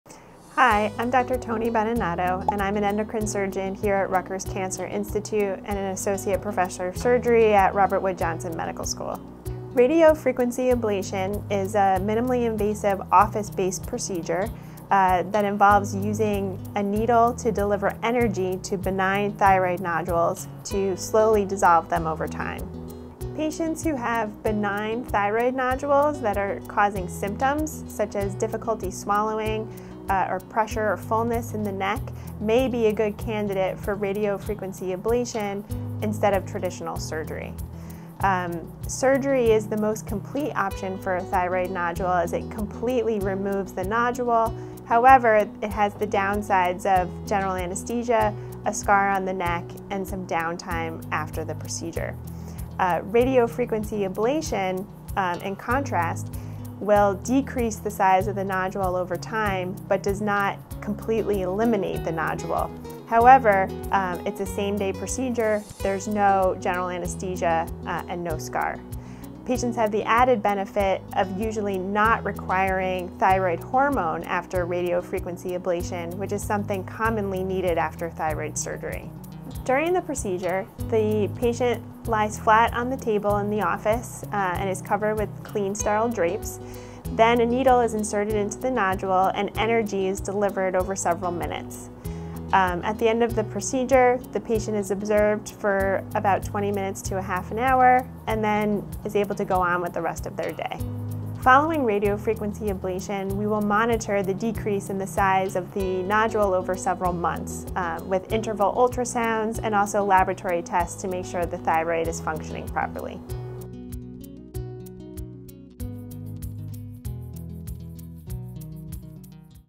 Thyroid Radiofrequency Ablation - Dr. Toni Beninato
What is thyroid radiofrequency ablation (RFA)?

Learn more about thyroid RFA, who is a good candidate, and what happens during this procedure with Rutgers Cancer Institute expert Dr. Toni Beninato.